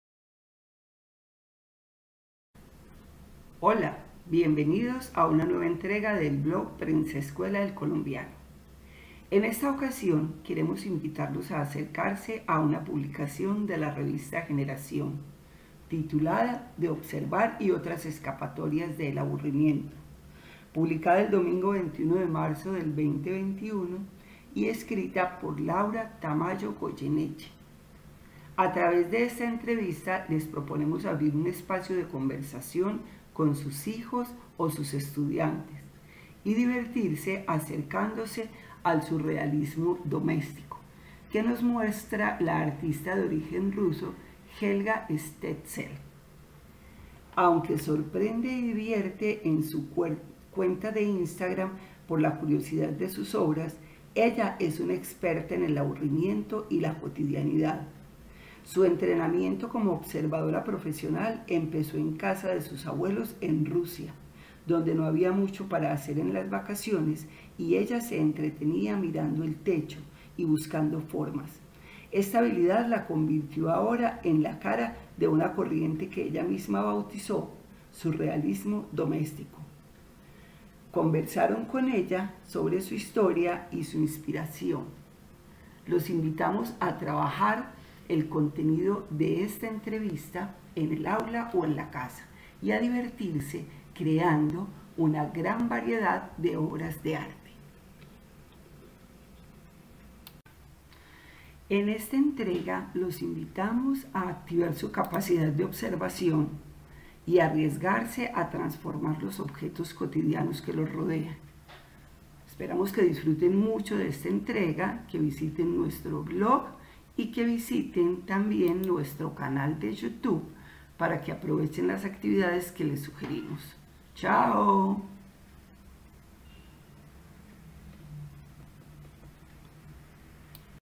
De observar y otras escapatorias al aburrimiento
De observar y otras escapatorias al aburrimiento: esta semana invitamos Helga Stentze, artista de origen ruso que divierte en la red por la creatividad de sus obras. Le gusta mucho mirar las cosas durante más tiempo de lo socialmente aceptado. Le gusta la observación visual que también llama meditación. Cuando ve algo, le gusta mirarlo bien, probablemente desde diferentes ángulos.